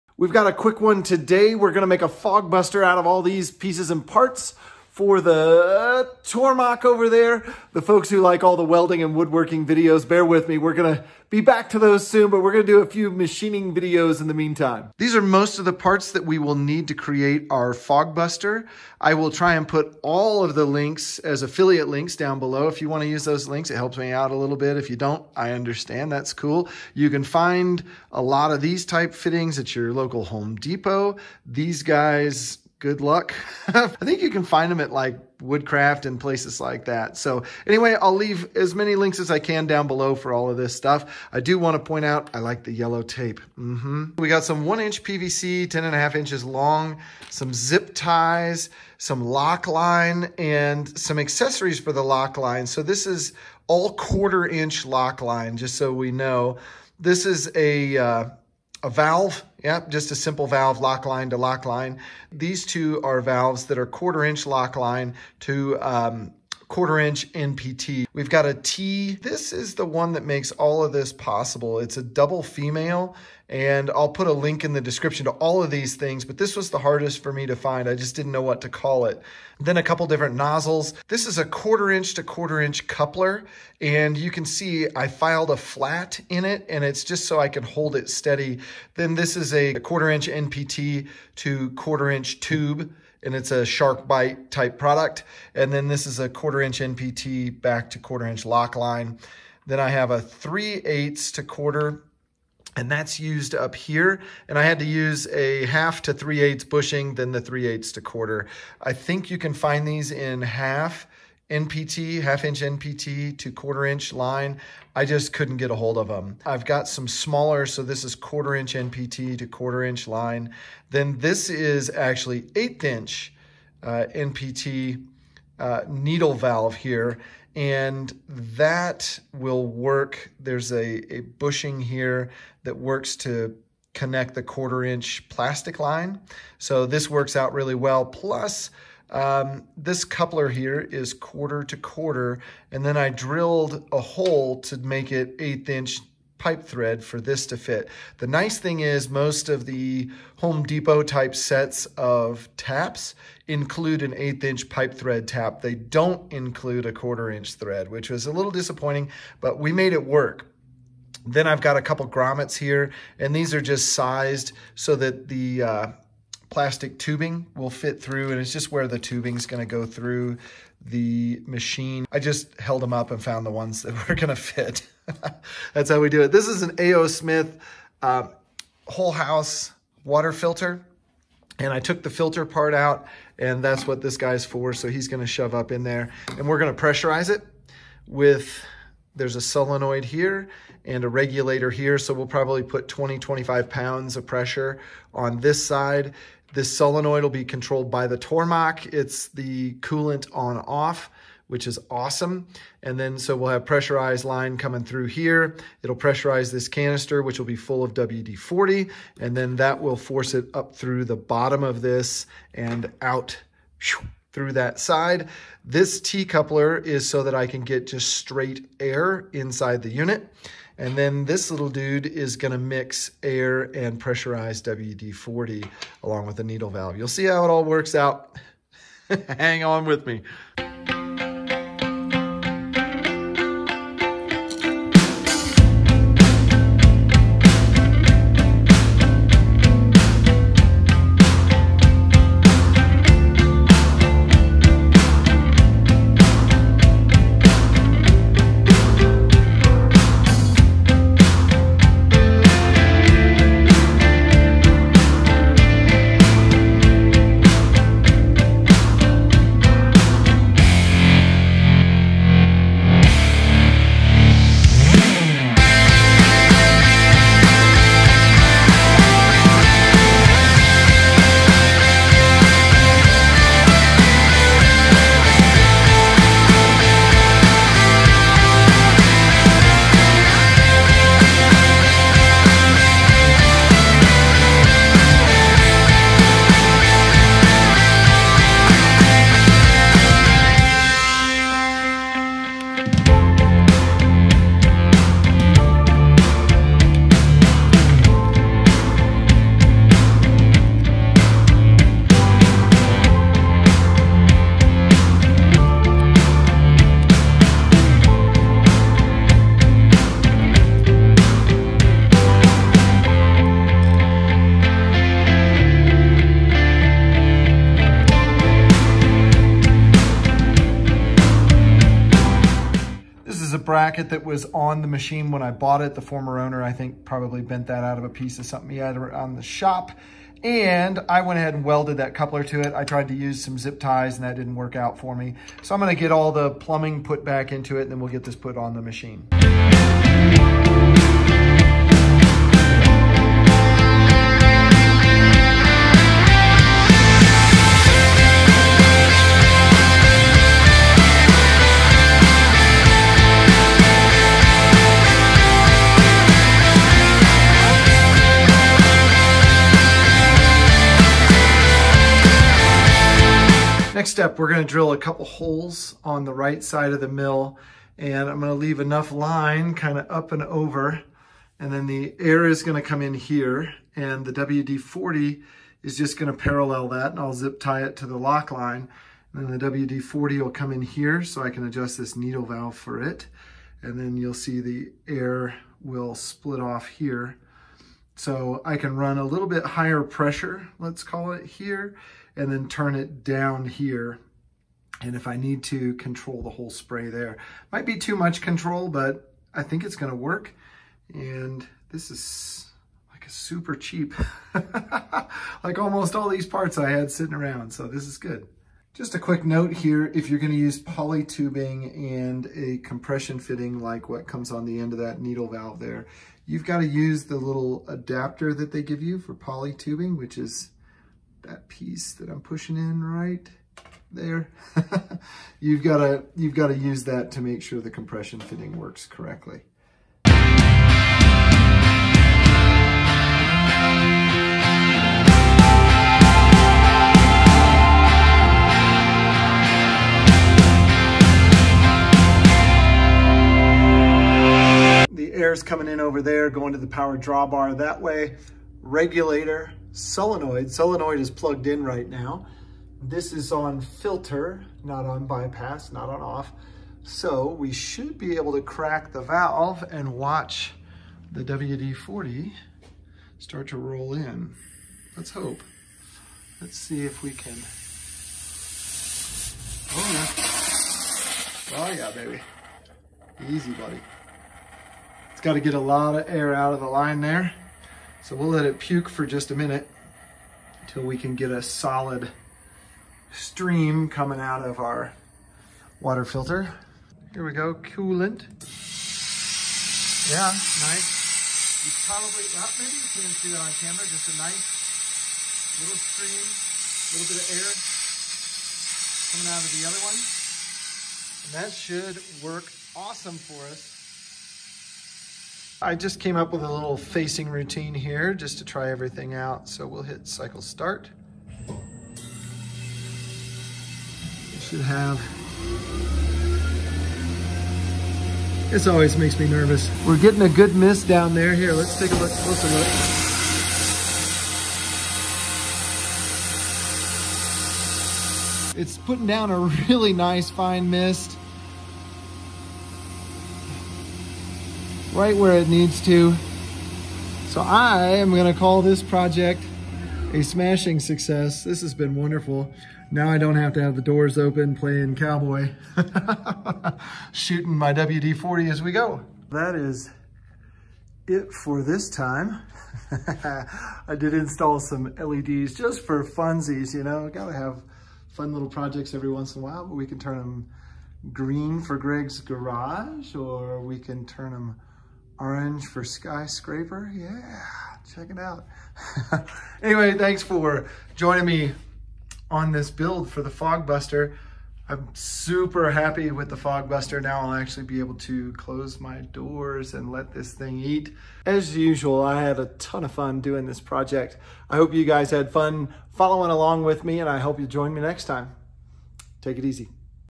SUPER CHEAP DIY Fogbuster Coolant System - Tormach 1100
BRASS PARTS
NEEDLE VALVE

PVC PARTS
1/4" NPT TO 1/4" POLY LINE (5 USED)
3/8" NPT TO 1/4" POLY LINE (2 USED)
1/2" NPT TO 1/4" POLY LINE
1/4" PUSH TO CONNECT T

OTHER PARTS
AO SMITH WHOLE HOUSE WATER FILTER
1/4" TO 1/4" COUPLER (2 NEEDED)
1/4" POLY TUBE
PRESSURE REGULATOR
1" PVC
YELLOW TAPE

CANON 80D
24-70 Lens
RODE VIDEO MIC PRO